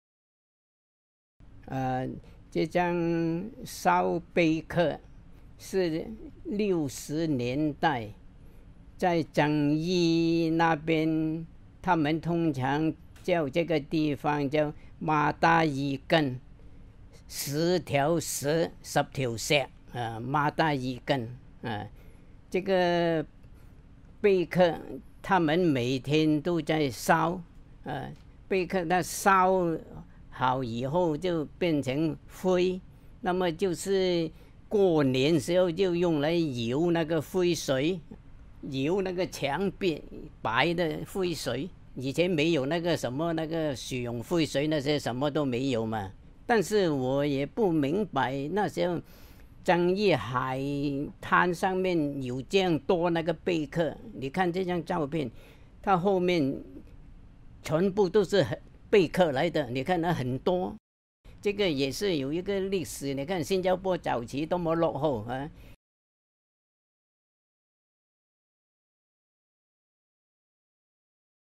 AUDIO CUTS: Hor Kwok Kin | 'Burning Seashells'
"This photograph of Burning Seashells was taken in the 1960s, at this location in Changi that was commonly known as Mata Ikan, or Changi Ten Mile. They were burning seashells everyday, reducing them to ashes. The ashes would be further processed and made into paint to paint the walls white during Chinese New Year; we didn’t have synthetic rubber paint back then. But I didn’t understand how there could be so many seashells along Changi Beach. Look at how many seashells there were in the background! This photograph also has a historical value, as it shows how backwards Singapore used to be." - Hor Kwok Kin

Featured artwork: Hor Kwok Kin, Burning Seashells, 1963, Gelatin Silver Print

Artwork is part of Through the Streets and the Darkroom: Photographic Work by Hor Kwok Kin, an exhibition organised by artcommune gallery. Through the Streets and the Darkroom showcases a nostalgic collection of over 40 black-and-white photographic images, which were captured and developed by the artist using darkroom techniques, and in limited editions, in the period of 1960s-80s. A commemorative monograph featuring 63 of the artist’s black-and-white photographic works accompanies the exhibition. Artworks are on view from 13 November till 30 November 2020, 12-7pm daily at the gallery.

Hor Kwok Kin (b. 1939, Ipoh) came to Singapore in 1958 and worked for over a decade at Loong Yick Kee Restaurant in Bugis before venturing out in the 1970s to open his own tze char stall. Mr Hor was a chef by occupation, but pursued photography with an unreserved passion. In his early days working at Loong Yick Kee Restaurant, he laboured from 11am to 1am daily, and without off days. With dogged determination, and by compromising the only window of rest he could afford, Mr Hor went onto the streets of Singapore and shuttled between ends of the island between 5:30-11am in the mornings to take photographs. As shoots were conducted in the early hours of the day, his images were often drenched in intense morning sunlight, giving rise to the dramatic backdrops for his subjects. Unable to afford the equipment and services that well-to-do photographers had access to, he converted the bathroom of the restaurant into a darkroom where he processed his photographs by mixing the chemicals from scratch.

Mr Hor’s art can be classified alongside the works of other modernist photographers, such as his friend and mentor Yip Cheong Fun. The thoughtful use of geometric elements in his compositions particularly recalls the latter’s influence. Deeply influenced by Henri Cartier-Bresson, he aimed in his practice to capture realist Singaporean scenes by seizing upon moments of working-class activities and everyday interactions among ordinary folks. As the period of Mr Hor’s practice coincided largely with the transition of a post-independent Singapore, he managed to archive in the process a variety of sites, professions and activities that once characterised life in Old Singapore.